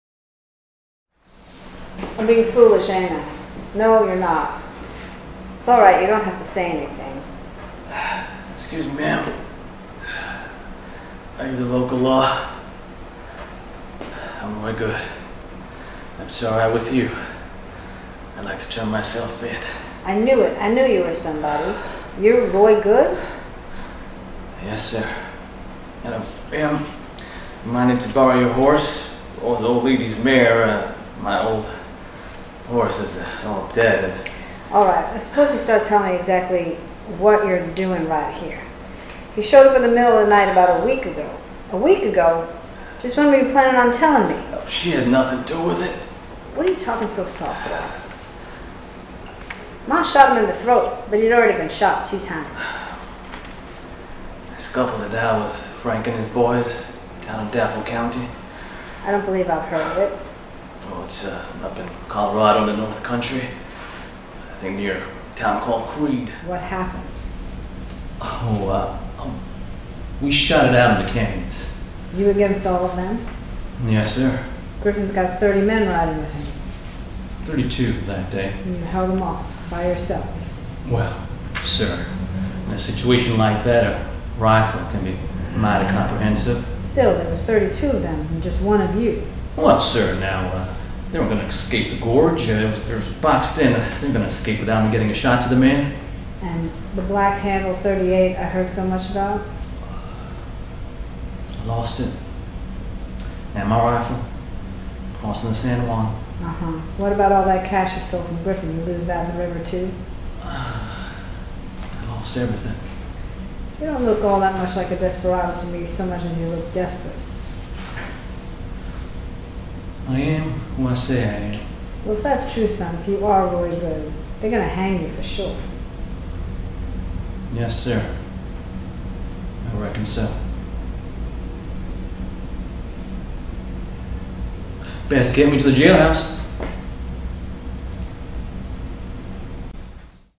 a scene from "Godless" performed by Kal Parekh
actor/writer Kal Parekh does a scene form Godless, ya' hear!